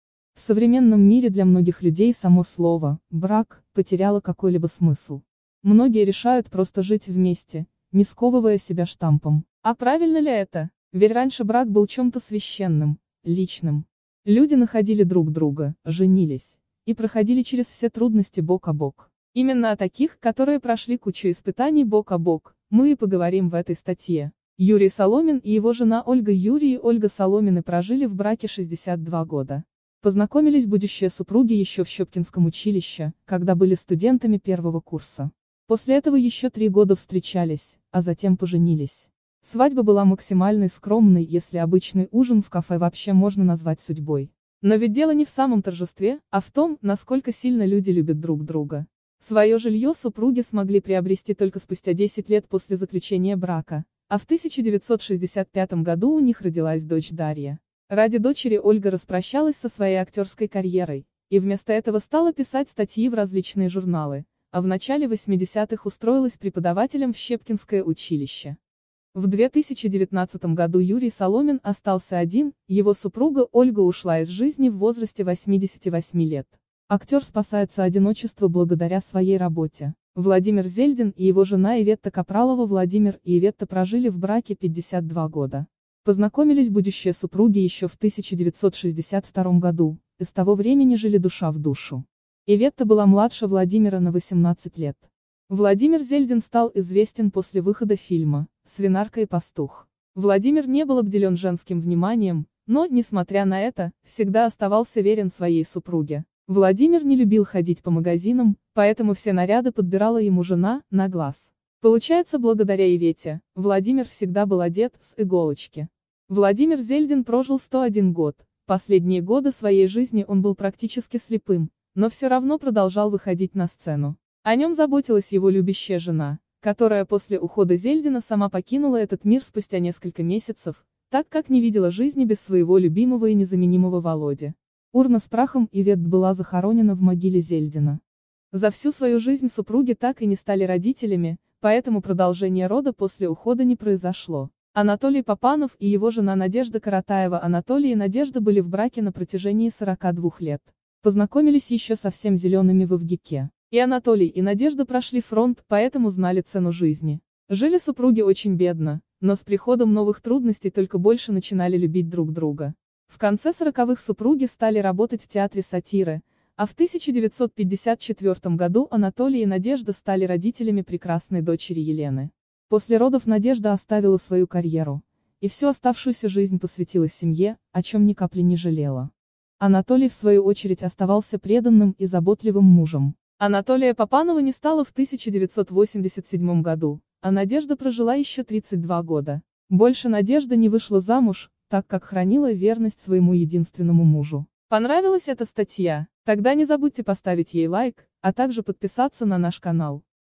И в огонь, и в воду: крепкие семейные пары в отечественном кинематографе
В современном мире для многих людей само слово «брак» потеряло какой-либо смысл. Многие решают просто жить вместе, не сковывая себя штампом. А правильно ли это? Верь раньше брак был чем-то священным, личным. Люди находили друг друга, женились, и проходили через все трудности бок о бок. Именно о таких, которые прошли кучу испытаний бок о бок, мы и поговорим в этой статье.  Юрий Соломин и его жена Ольга  Юрий и Ольга Соломины прожили в браке 62 года. Познакомились будущие супруги еще в Щепкинском училище, когда были студентами первого курса. После этого еще три года встречались, а затем поженились. Свадьба была максимальной скромной, если обычный ужин в кафе вообще можно назвать судьбой. Но ведь дело не в самом торжестве, а в том, насколько сильно люди любят друг друга.